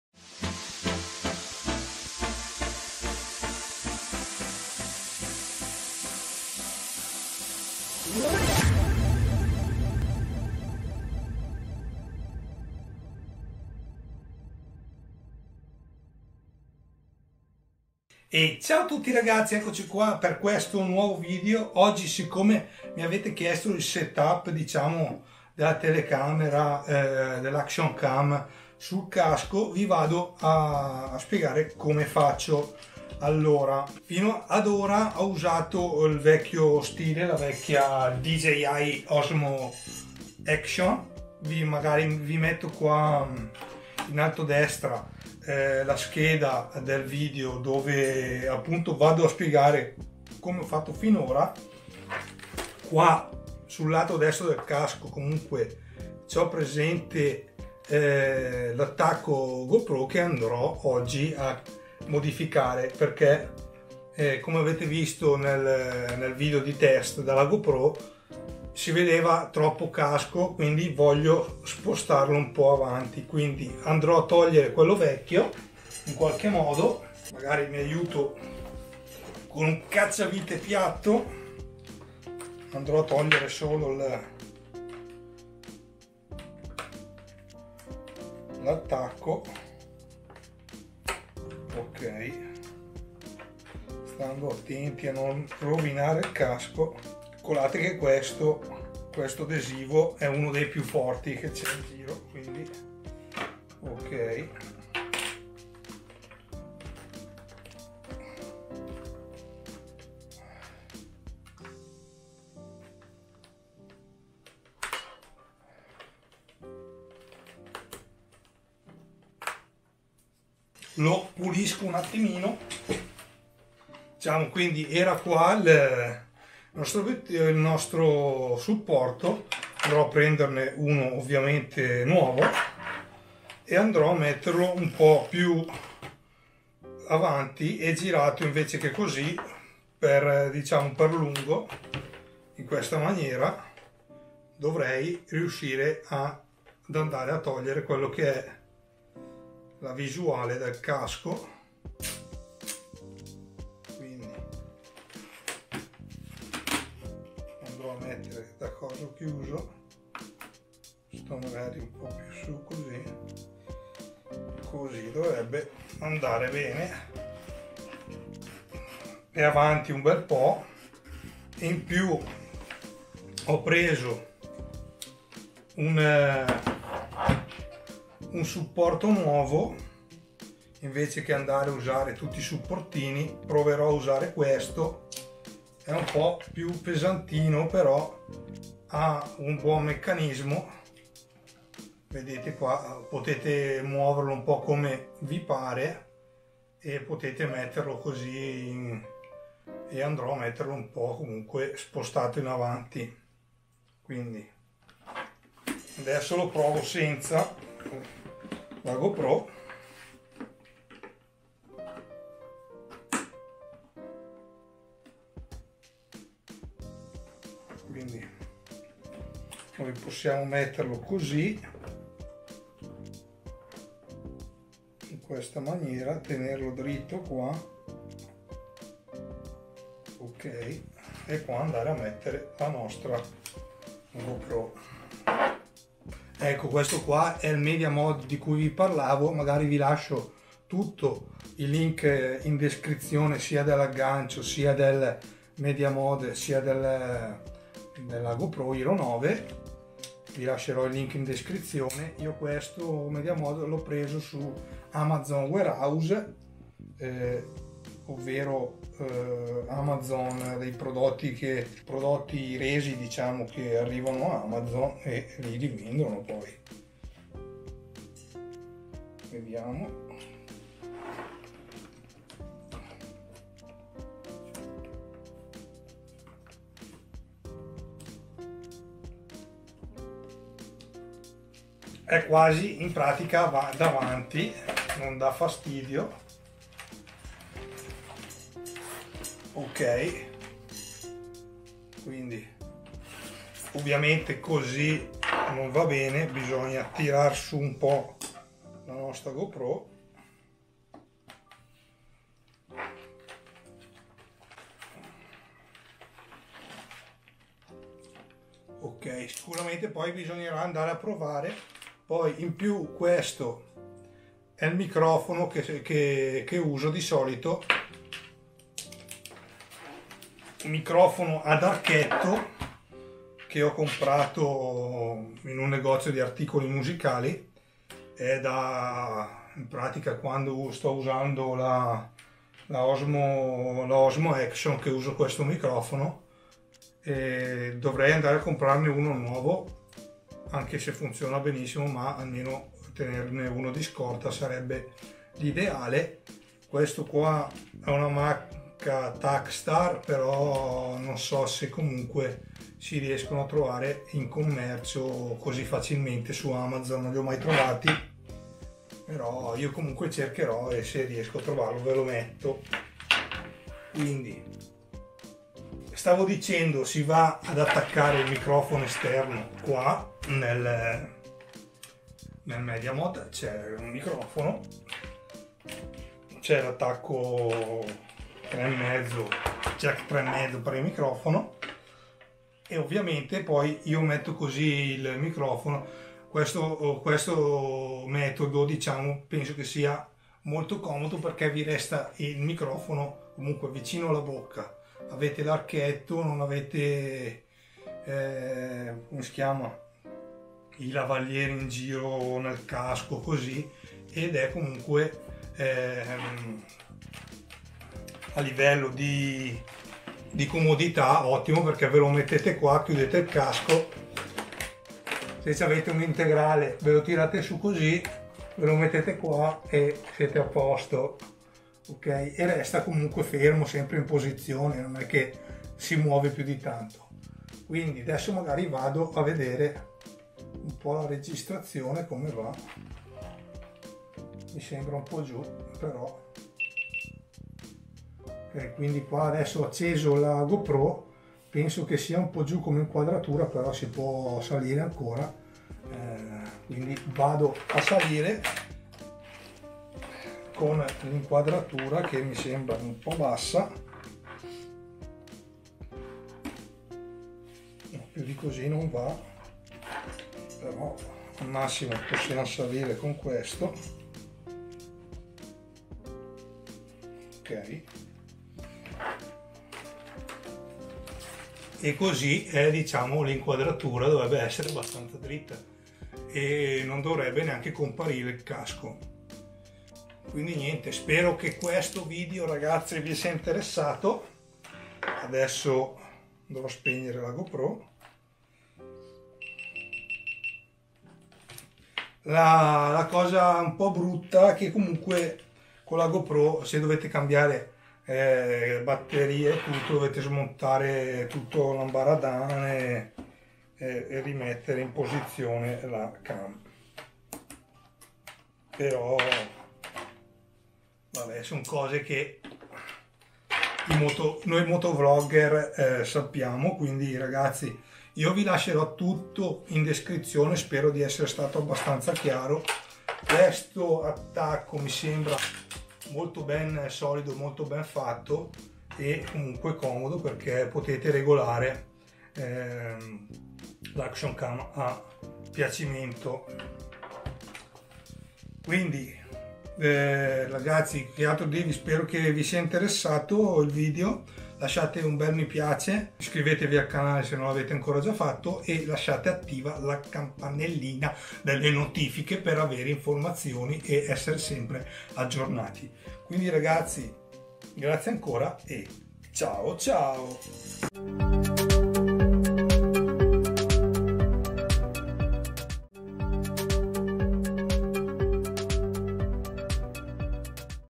SETUP MOTOVLOG
In questo video vi porto il mio setup da

questi gli accessori che utilizzo:
Gopro hero 9 black
media mod
supporto gopro

ACCESSORI VARI

ABBIGLIAMENTO